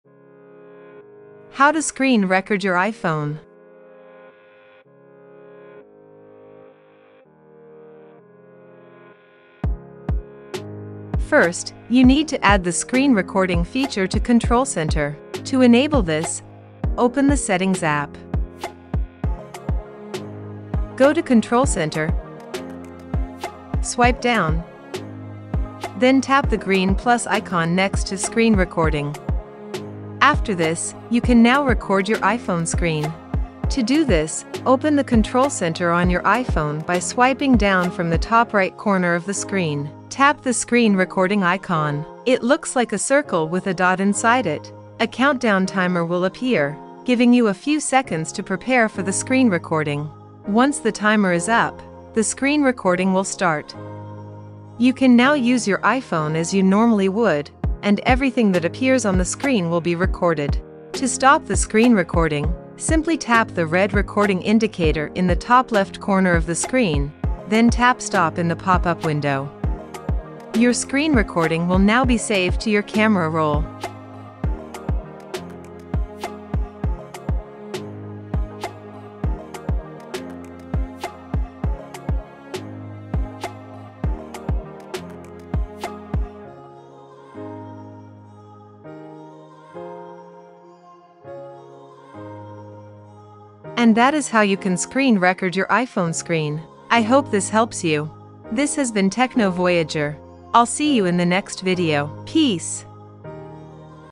How to screen record your iPhone
This video tutorial shows you how you can screen record your iPhone.